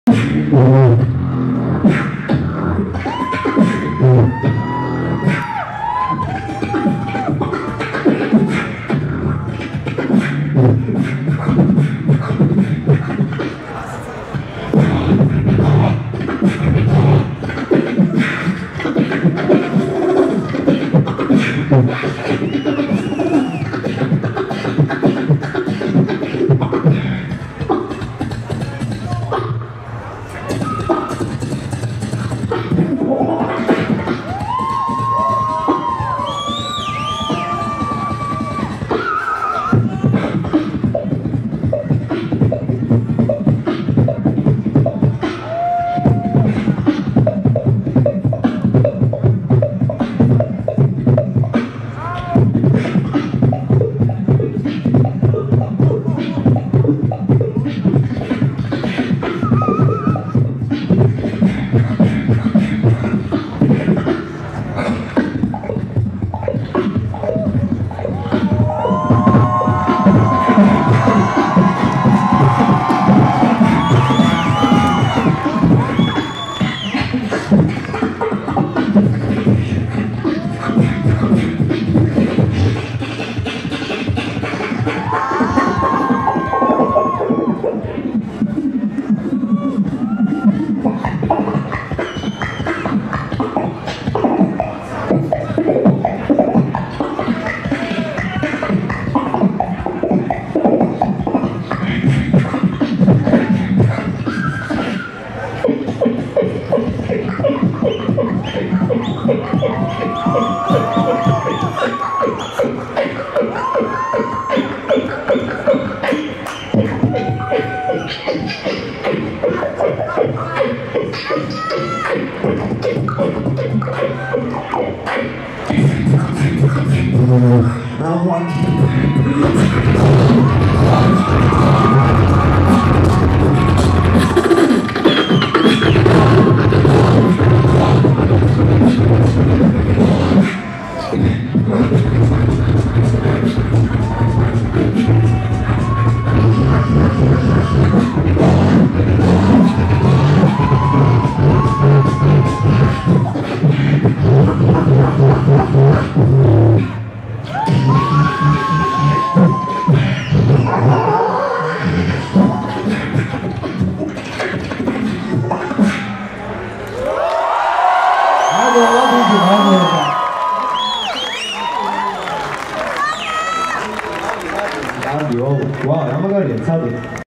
BBX AWP SHOWCASE AT SKATE PARK ( HD / 1080p )
GEORGIA / TBILISI ( SKATE PARK )

BBOY AWP FROM PARANORMAL CREW
BBX AWP FROM S4H CREW